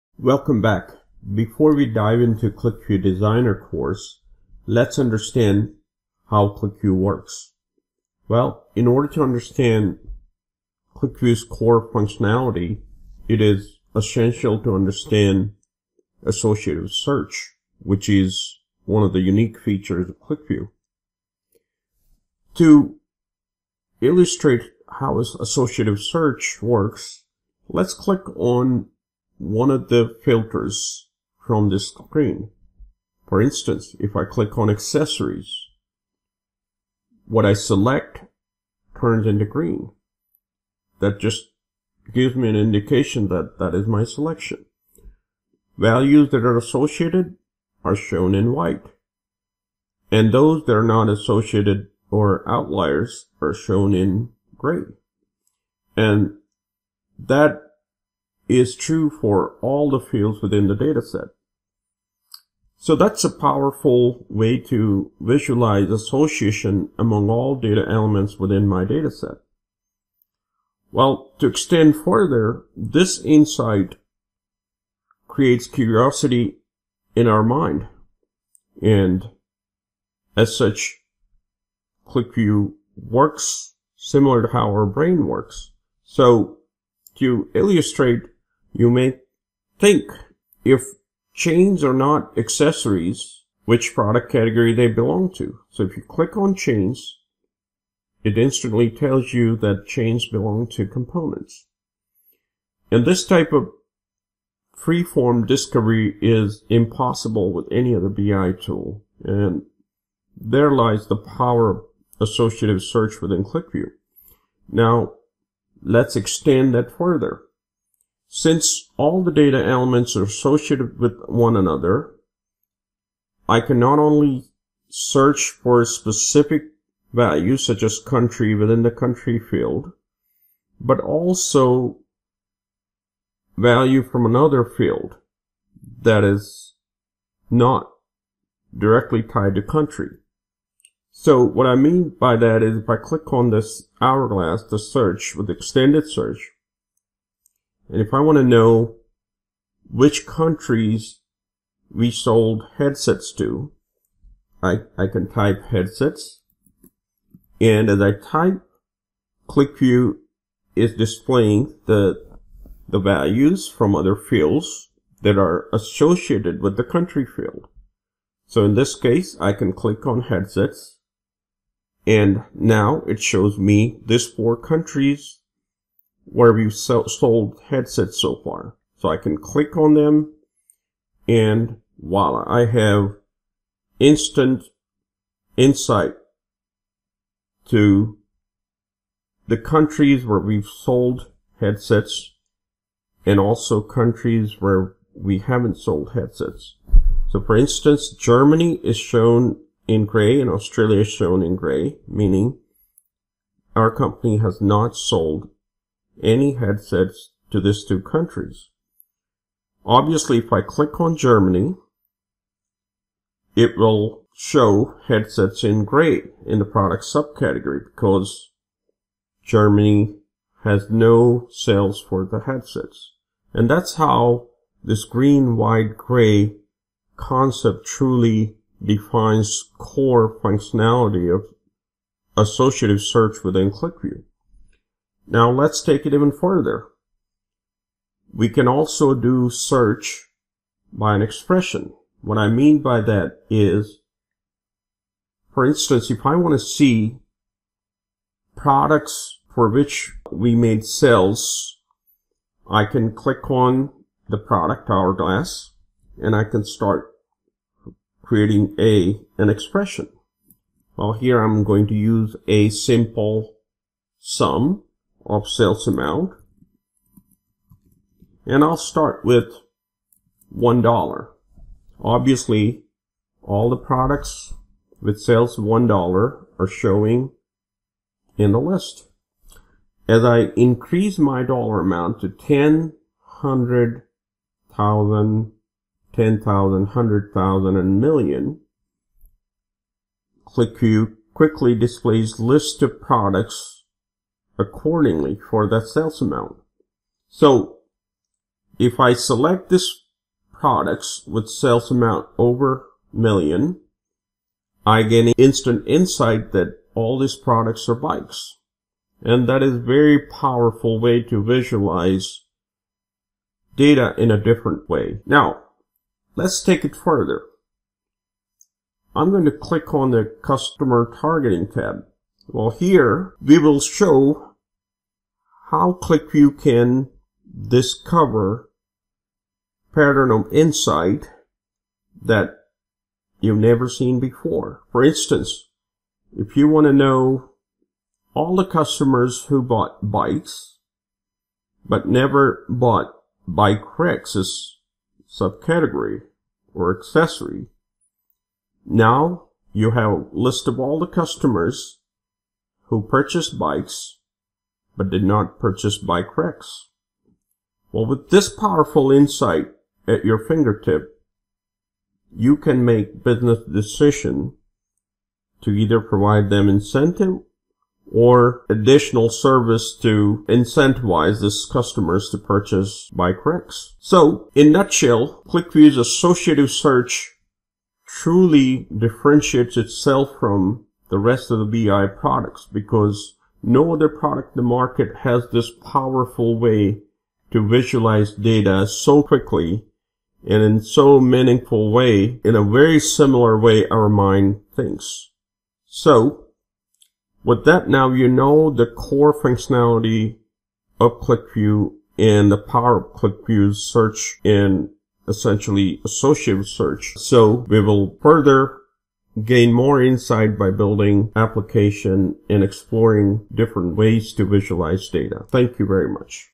QlikView Training: QlikView Designer Course
This QlikView training course will teach you how to build a QlikView application step-by-step using data visualization best practices.

Use this URL to enroll in my udemy course for just $20

Learn all the advanced features of QlikView 11.2 including Set Analysis and Alternate States for comparative analysis.